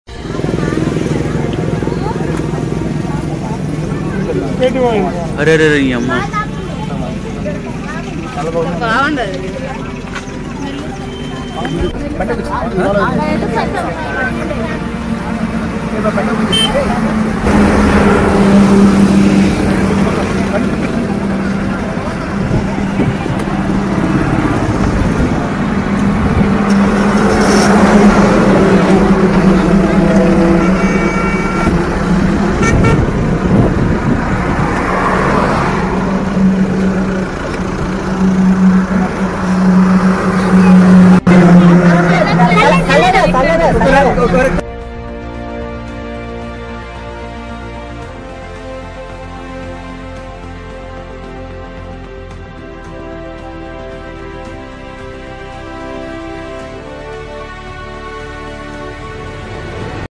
పాపం పసిపాప...అమ్మ వదిలేసింది... ఎస్సై అక్కున చేర్చుకున్నాడు
మహబూబ్ నగర్ జిల్లా దేవరకద్ర మండలం కాకతీయ స్కూల్ సమీపంలో వెంగమాంబ దాబ ముందు ఉన్న బెంచి పై 20 రోజుల ఆడ పసికందును వదిలివెళ్లిన గుర్తుతెలియని తల్లి దండ్రులు ...విషయం తెలుసుకున్న  దేవరకద్ర ఎస్ ఐ భగవంత రెడ్డి హుటాహుటిన అక్కడికి చేరుకొని తానే స్వయంగా పసికందును  తీసుకొని వచ్చి దేవరకద్ర  వైద్య చేయించి ఐసీడీఎస్ అధికారులకు అప్పగించిన దేవరకద్ర ఎస్సైకి స్థానికులు అభినందనలు తెలిపారు.